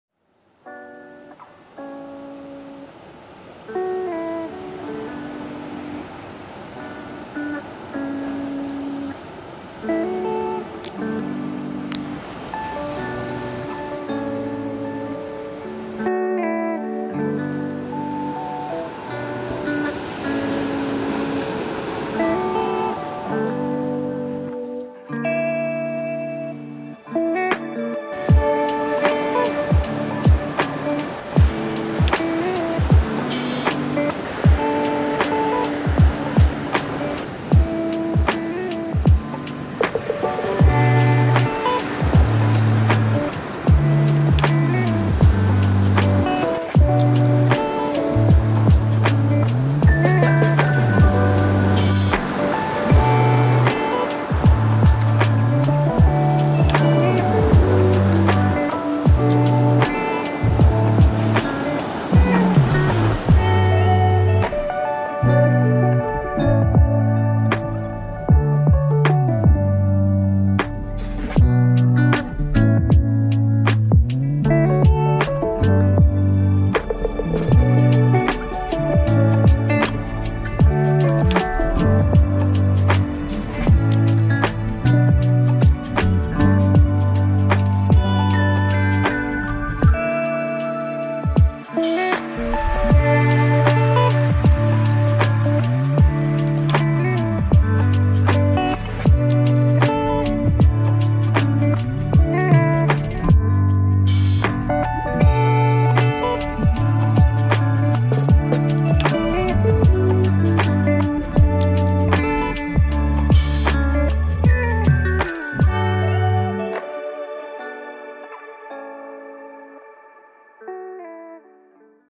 Eos M + Kit lens 15-45mm - 14 Bit Raw video test, Bali 2021
EOS-M (Magic Lantern)
Here is a mini review of Canon Eos M and 15-45mm IS STM for EOS M series cameras. I must say, I was quite surprised by the build quality and the footage from this cheap zoom kit.
Это тест камеры Canon Eos M(с прошивкой  magic lantern) and 15-45mm IS STM это кайфовый комплект супер бюджетной камеры которая дает качество кинокамеры
MCM Rewire 1080
Resolution: 1736x976 [16:9] Upscaled to [16:9] 3840x2160
ISO 100
Shutter 1/50
25 fps